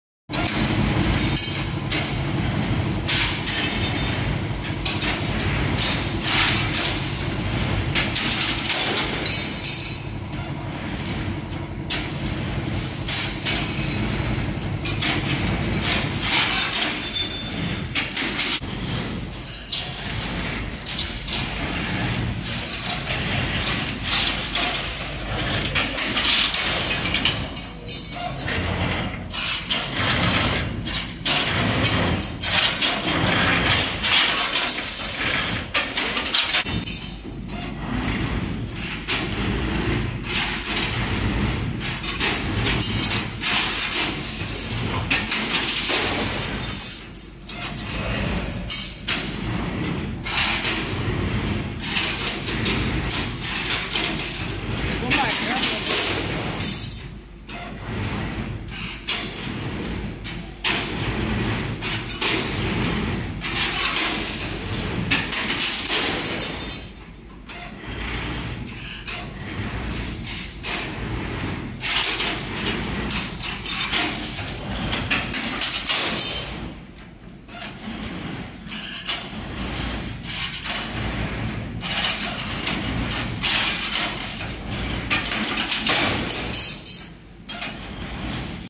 16mm CNC stirrup bending machine
16mm CNC stirrup bending machine being used in customer's factory